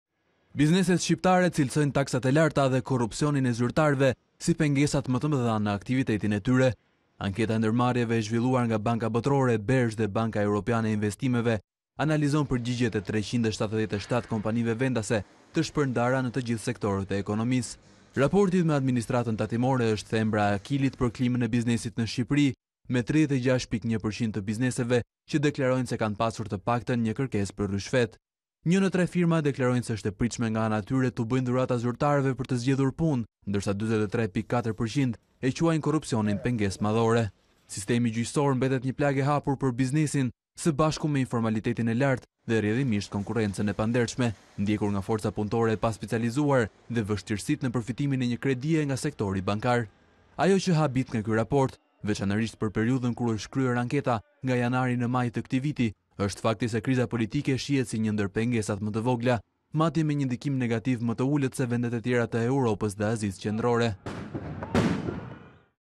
Shqetësimet e biznesit, plagë taksat e larta, korrupsioni dhe gjykatat
Bizneset shqiptare cilësojnë taksat e larta dhe korrupsionin e zyrtarëve si pengesat më të mëdha në aktivitetin e tyre. Anketa e Ndërmarrjeve e zhvilluar nga Banka Botërore, BERZH dhe Banka Europiane e Investimeve analizon përgjigjet e 377 kompanive vendase të shpërndara në të gjithë sektorët e ekonomisë.

Raporti me administratën tatimore është thembra e Akilit për klimën e biznesit në Shqipëri, me 36.1% të biznesit që deklaron se kanë pasur të paktën një kërkesë për ryshfet. Një në tre firma deklarojnë se është e pritshme nga ana e tyre t’u bëjnë dhurata zyrtarëve për të zgjidhur punë. Ndërsa 43.4% e quajnë korrupsionin pengesë madhore.